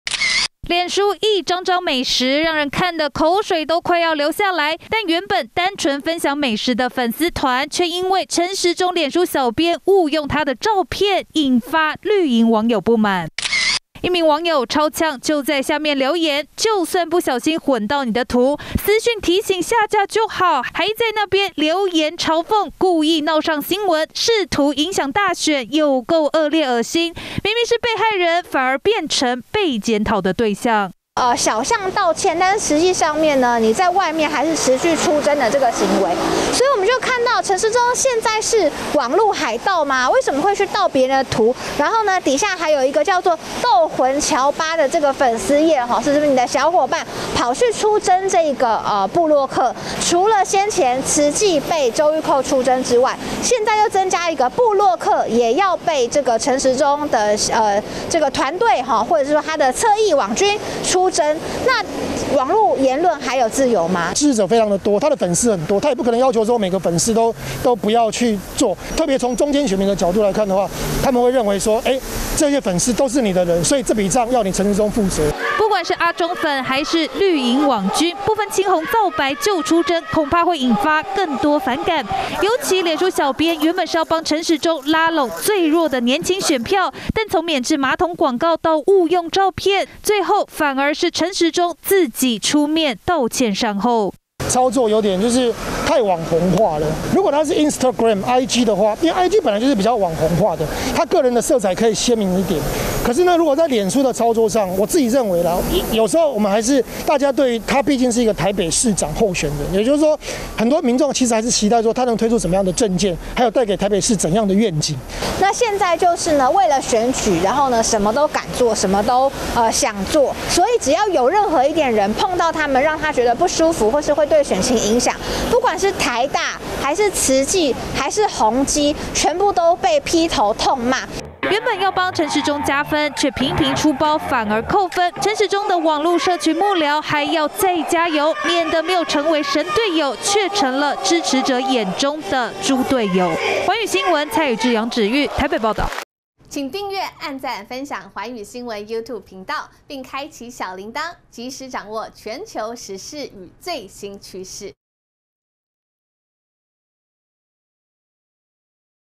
陳時中空戰搶票滑鐵盧 滷肉飯.免治馬桶接連凸槌？@globalnewstw
年底選戰倒數74天！陳時中臉書照片鬧出盜圖爭議，也讓他親自出來道歉，原本以為事情結束了，沒想到綠營側翼，還對被盜圖的部落客留言嗆聲，再次點燃網友怒火，就被外界認為是豬隊友！回顧陳時中最近曝光的政策，從免治馬桶政見影片出問題，現在又有盜圖風波，原本想發揮創意打空戰，極力拉攏年輕選票，但現在還沒加分已經先失分。